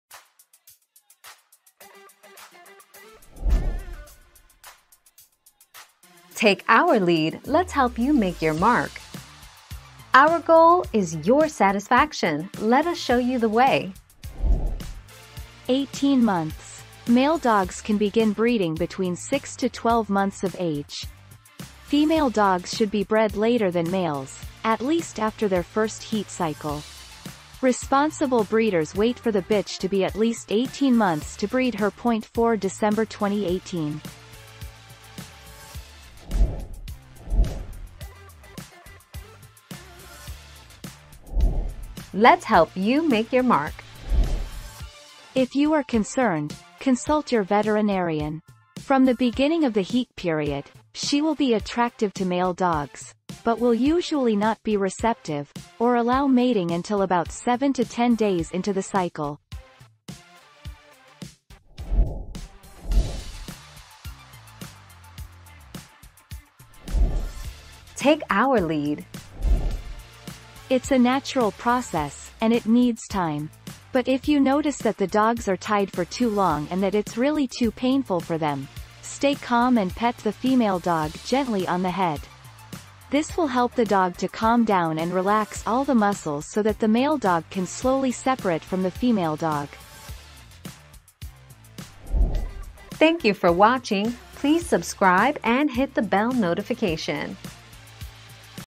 How long should you wait to breed a female dog?
00:00 - How long should you wait to breed a female dog?
00:38 - How many days after a dog starts bleeding is she ready to breed?
01:03 - How do you separate two dogs stuck together?

Laura S. Harris (2021, April 25.) How long should you wait to breed a female dog?
    AskAbout.video/articles/How-long-should-you-wait-to-breed-a-female-dog-240705